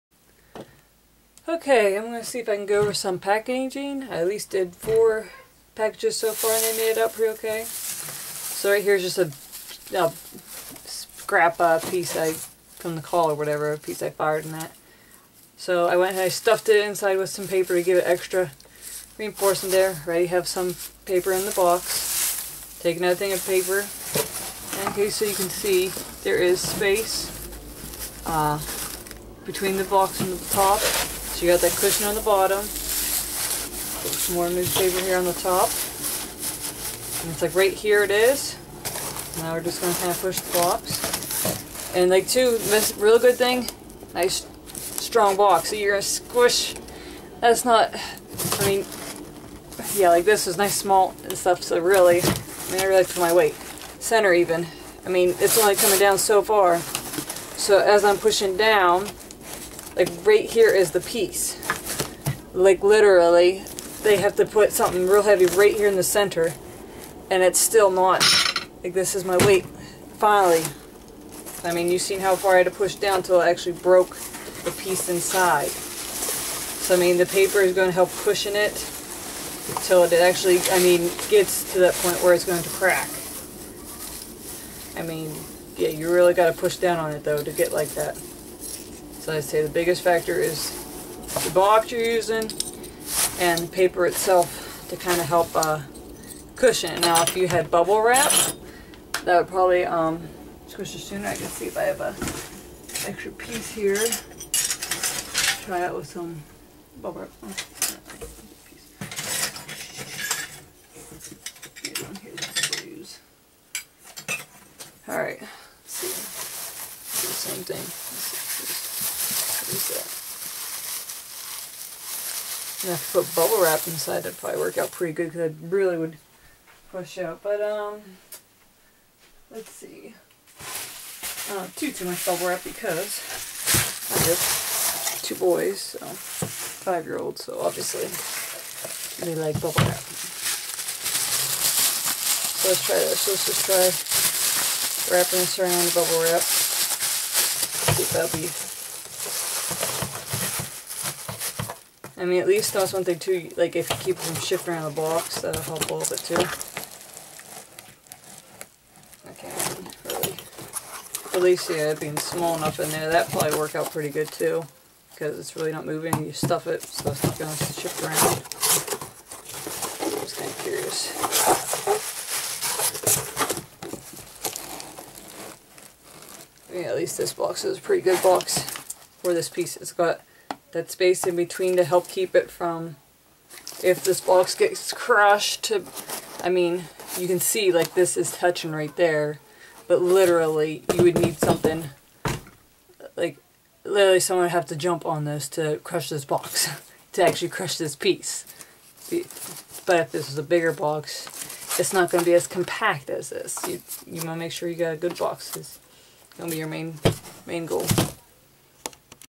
packing ceramics
A video of how I pack ceramics